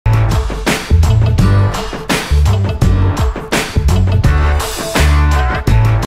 The Philips All-in-One Cooker
Save time and cook a variety of delicious meals in 30 minutes with the Philips All-in-One Cooker.

With 35 presets already in place, you can take the hassle out of home cooking with our All-in-One Cooker.